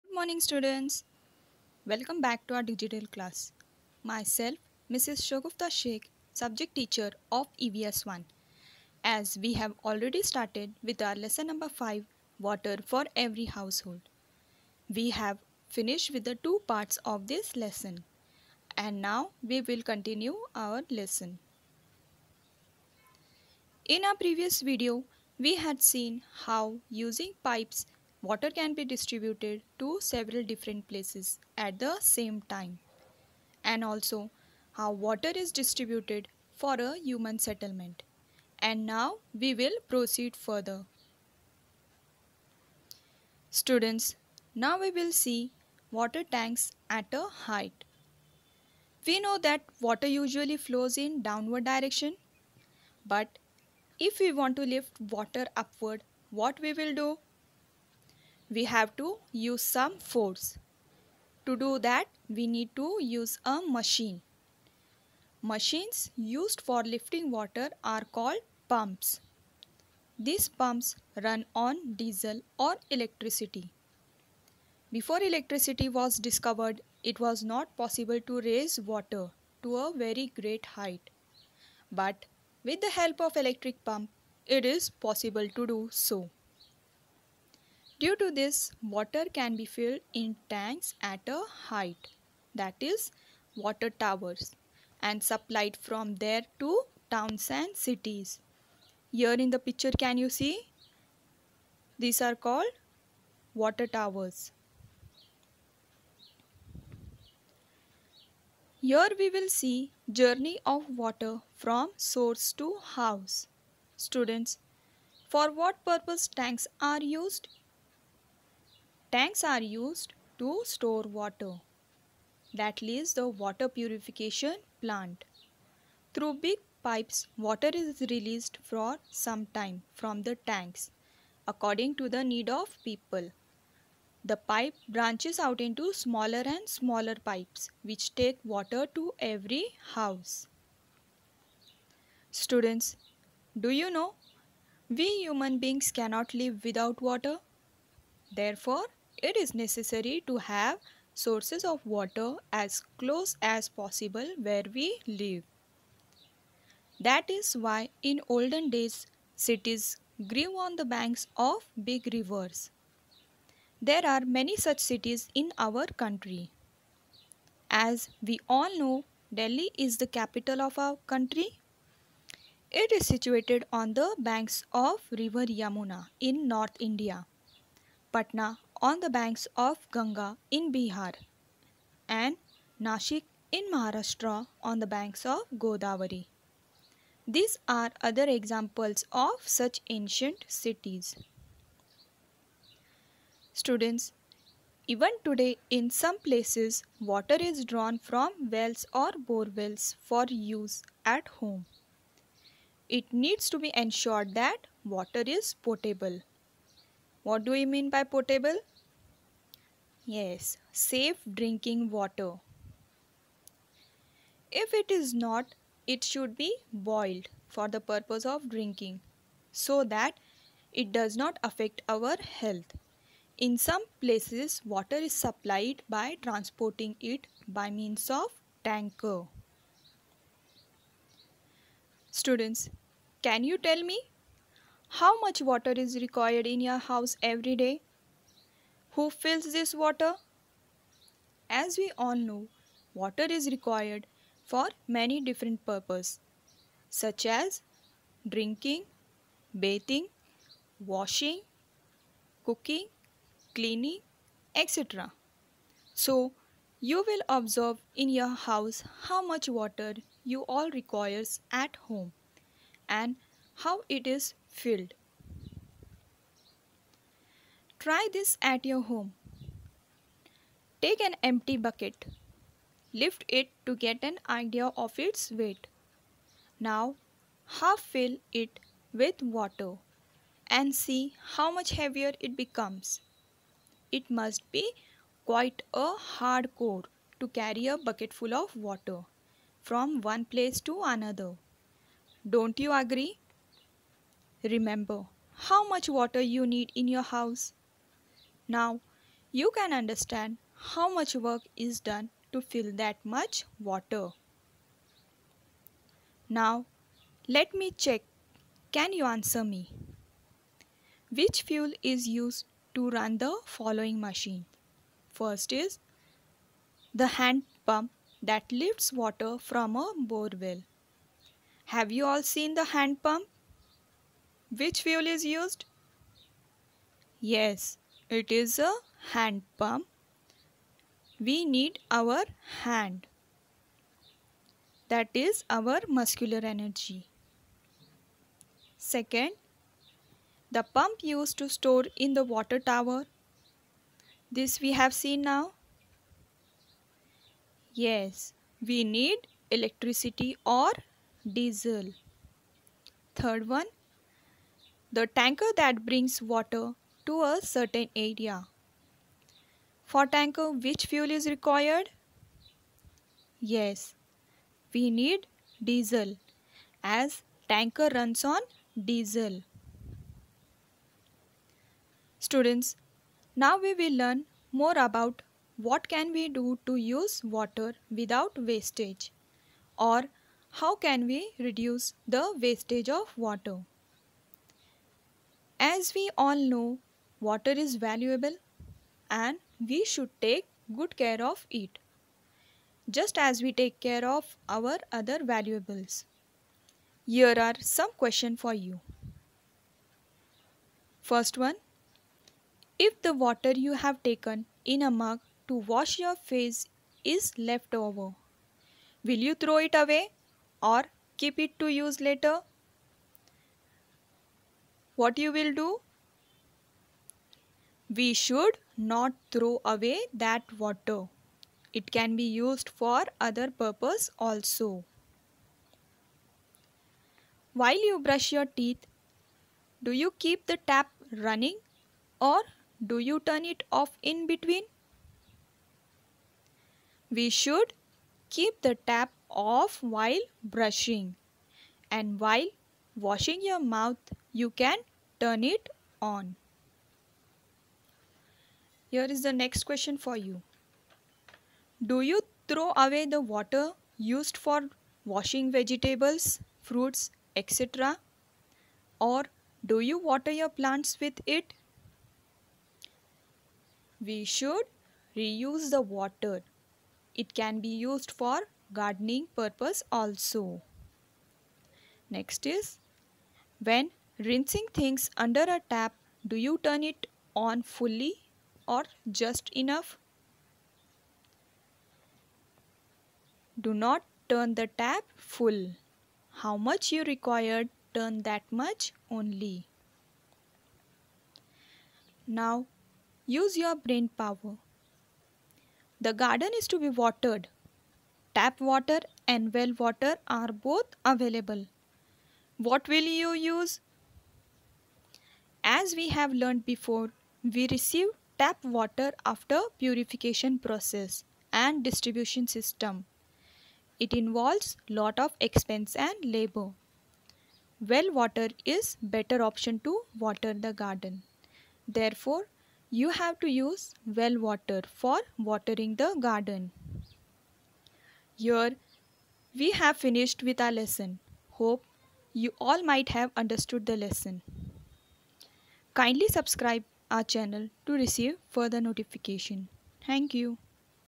Std -IV. Subject-Evs I. Topic - Water for every House
Std-IV   Subject-EVS I
L.no-5 Water For Every Household
Description:- This video describes about water tanks at a height and journey of water from source to house .